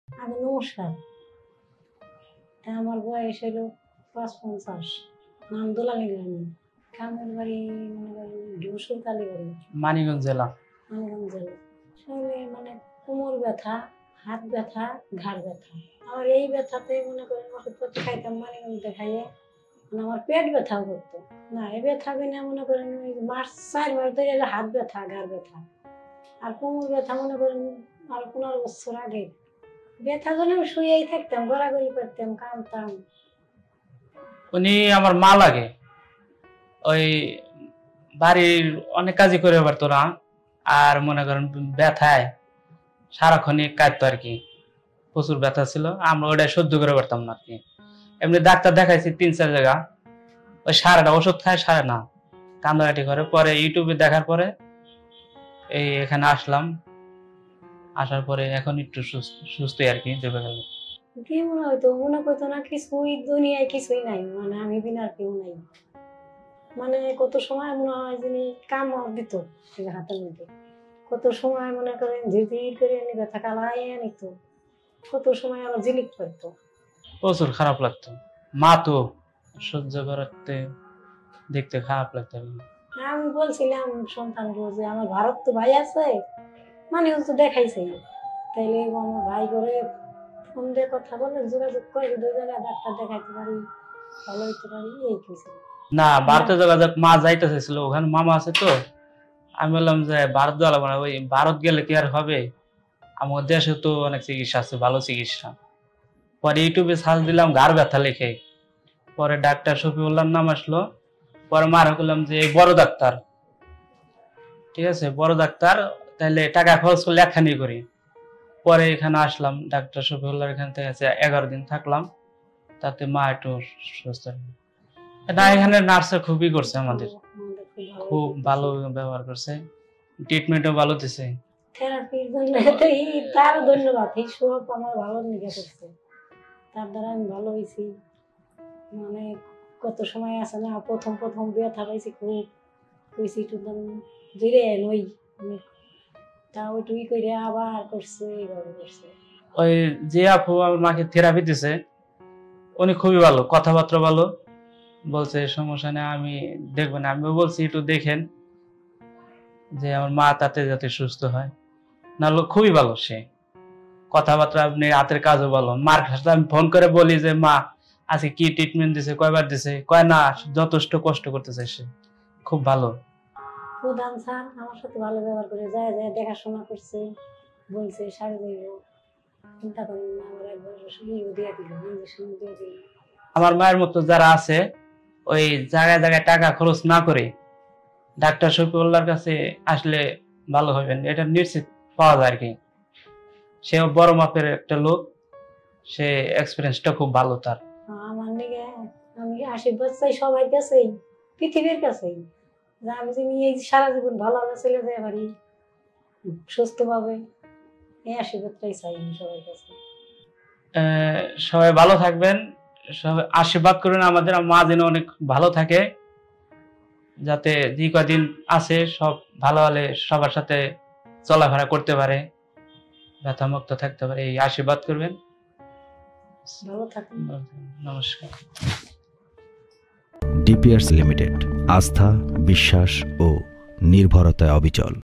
কোমর, ঘাড় ও হাটু ব্যথার রোগী আলহামদুলিল্লাহ সুস্থ | DPRC Hospital | Dr.Md.Shafiullah Prodhan | PLID
✺আমাদের  এর ঠিকানাঃ-
DPRC
12/1 Ring Road, shyamoli Dhaka-1207, Bangladesh.
(ডিপিআরসি , 12/1  রিং রোড শ্যামলি ঢাকা-১২০৭ বাংলাদেশ)